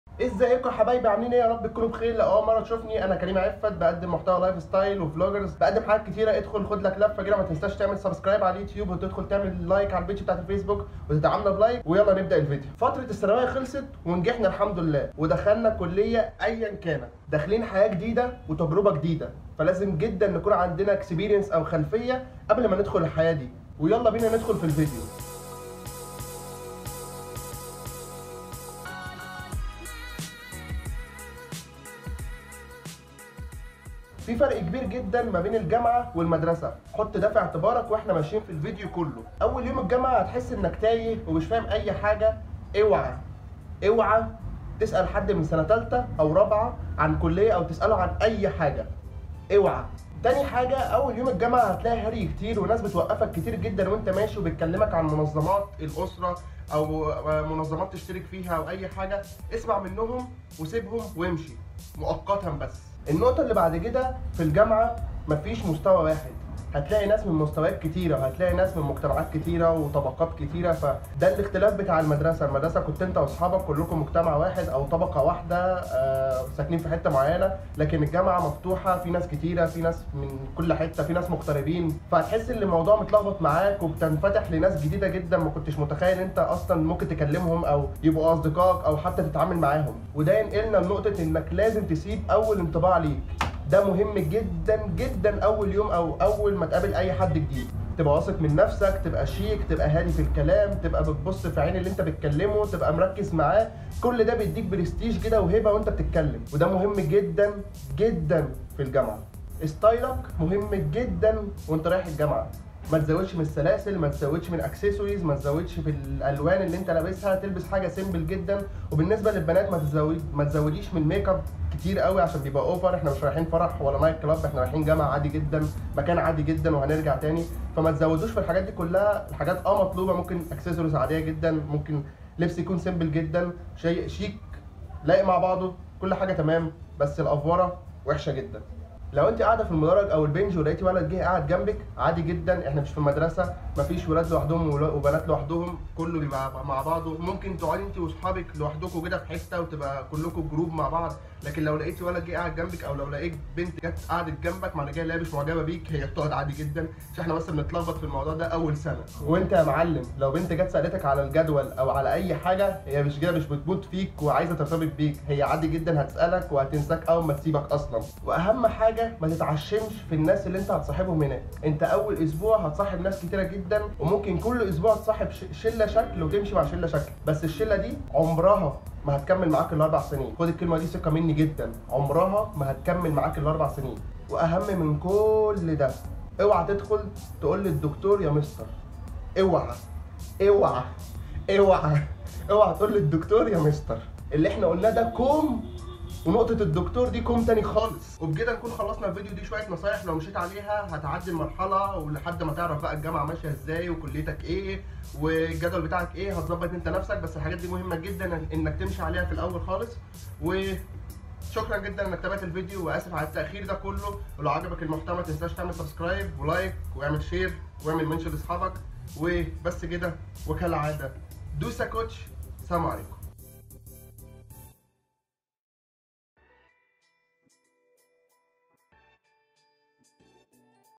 كل حاجة محتاج تعرفها عن اول يوم جامعة و ازاي تتعامل _ كريم عفت _ _ Karim Afat
اول يوم الجامعه بتبقي تايه ومش عارف تعمل ايه وبتبقي متسوح ...
الفيديو ده هيفهمك حياه الجامعه ماشيه ازاي !!